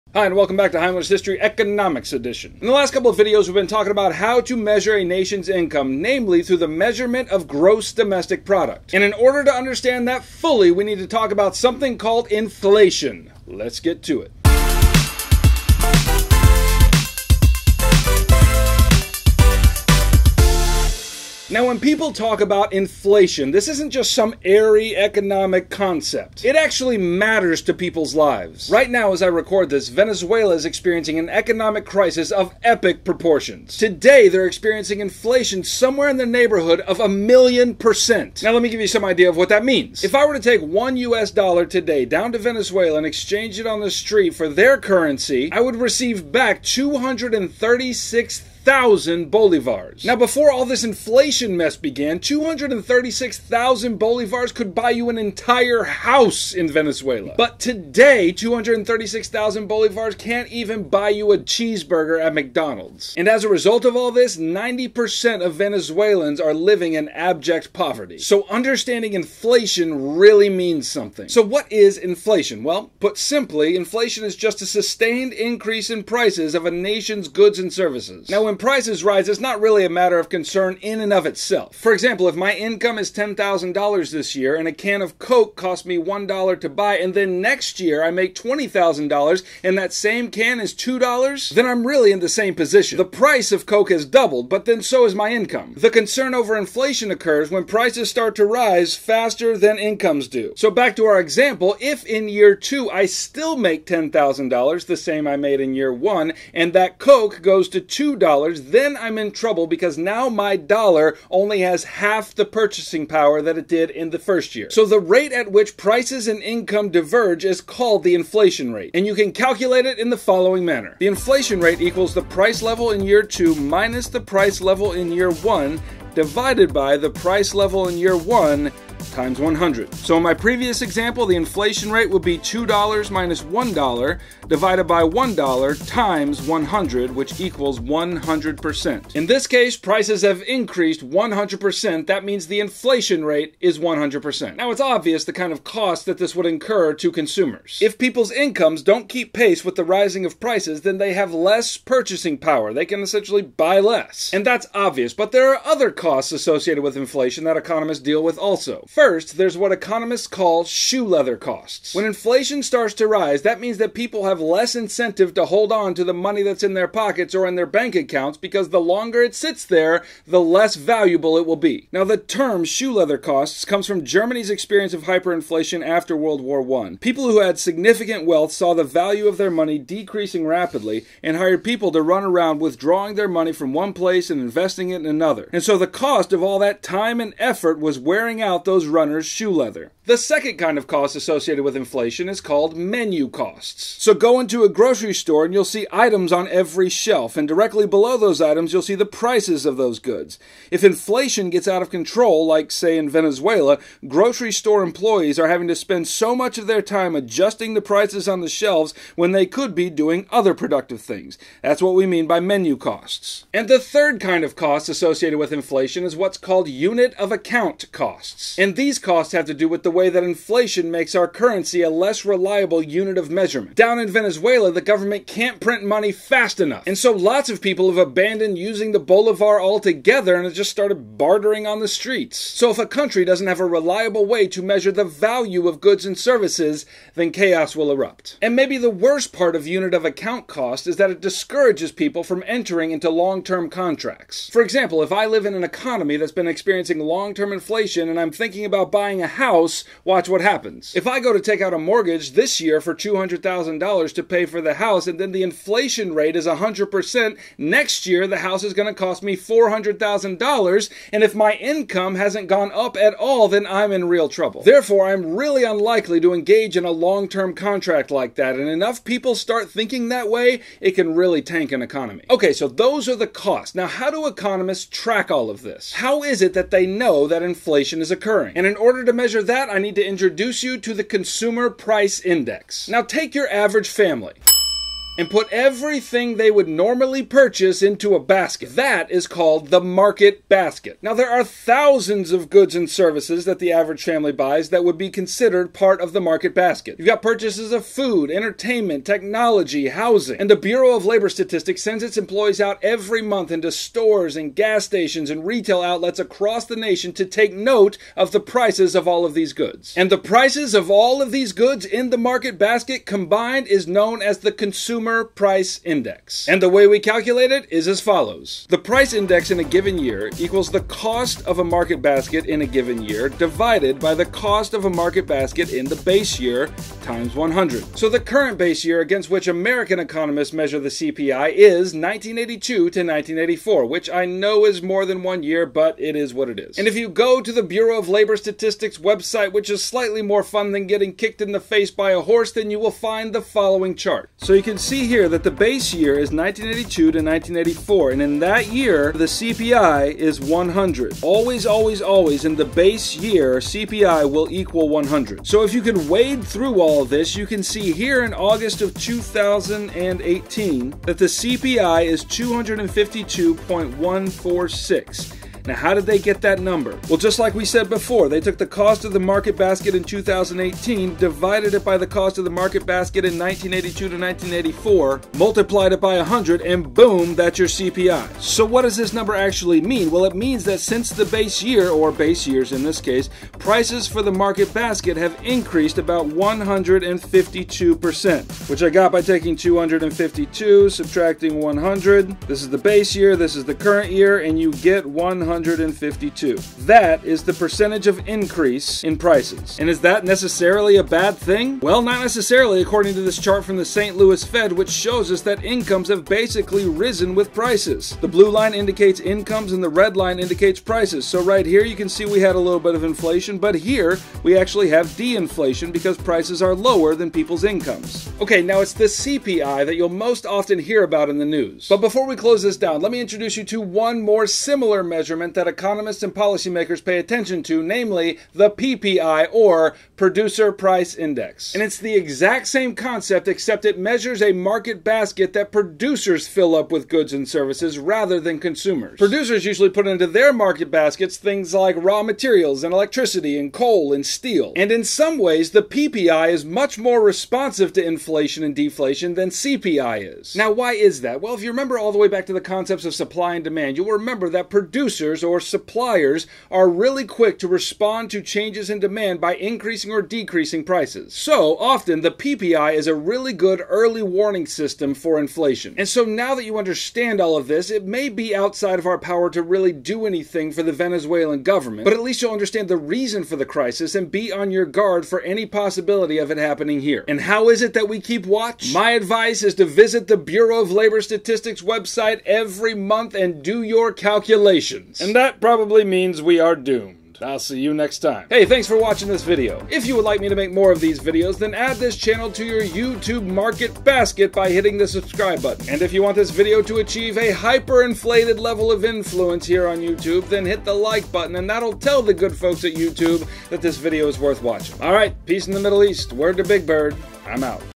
Inflation & the Consumer Price Index (CPI)—AP Macroeconomics Review
In this video Heimler explains what inflation is and how to measure it: the inflation rate. And one of the most reliable measures (though not perfect) for measuring inflation year over year is the Consumer Price Index (CPI). The Consumer Price Index measures the price of a market basket of goods that an average urban family consumes and compares it to a base year to get some idea of how prices have changed.

If you haven’t done enough economic calculations lately, then your cup shall be full after this video.

Inflation and the Consumer Price Index falls under Unit 2 in the AP Macroeconomics curriculum.